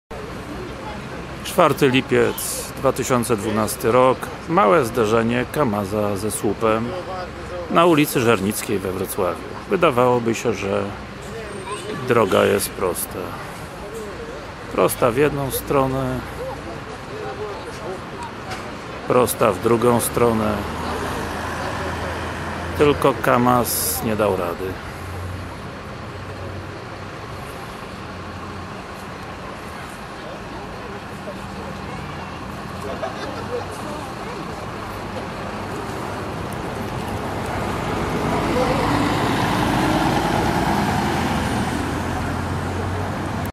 Ciężarówka wpadła na słup
Na ulicy Żernickiej we Wrocławiu ciężarówka wpadła na słup. Odcinek drogi był prosty i nie wiem jaka była przyczyna tego wypadku. Na miejsce przyjechała straż pożarna, aby zabezpieczyć kable elektryczne, które zostały zerwane. Przez kilkanaście godzin okoliczne domy pozbawione były dopływu energii elektrycznej, Ruch samochodowy w obie strony ulicy został zrobiony wahadłowo.